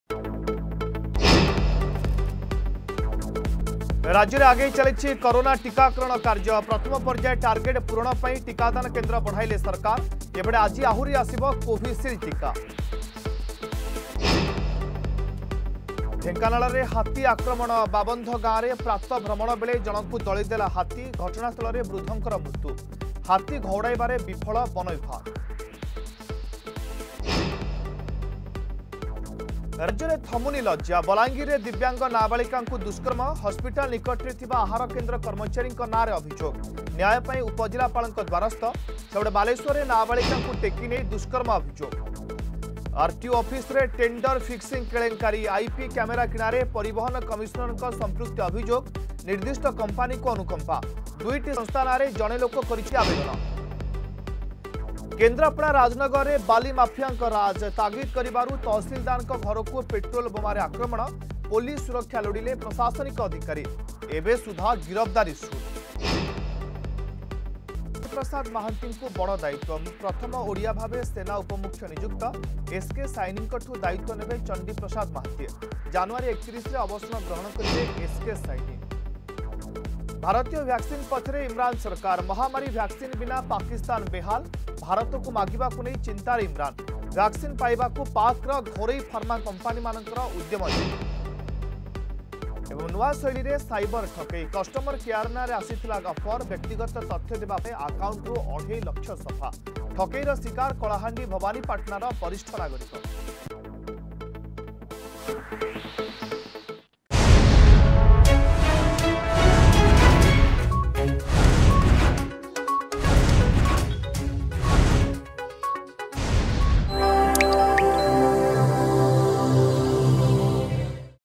Headlines 9am
ରାଜ୍ୟରେ ଆଗେଇ ଚାଲିଛି କରୋନା ଟିକାକରଣ କାର୍ଯ୍ୟ...
ଢେଙ୍କାନାଳରେ ହାତୀ ଆକ୍ରମଣ...
ରାଜ୍ୟରେ ଥମୁନି ଲଜ୍ଜା.
ଆରଟିଓ ଅଫିସରେ ଟେଣ୍ଡର ଫିକ୍ସିଂ କେଳେଙ୍କାରୀ...
କେନ୍ଦ୍ରାପଡା ରାଜନଗରରେ ବାଲି ମାଫିଆଙ୍କ ରାଜ୍ ...
ଓଡିଆ ପୁଅ ଚଣ୍ଡୀ ପ୍ରସାଦ ମହାନ୍ତିଙ୍କୁ ବଡ ଦାୟିତ୍ୱ.
ଭାରତୀୟ ଭ୍ୟାକ୍ସିନ୍ ପଛରେ ଇମ୍ରାନ ସରକାର .
ନୂଆ ଶୈଳୀରେ ସାଇବର୍ ଠକେଇ...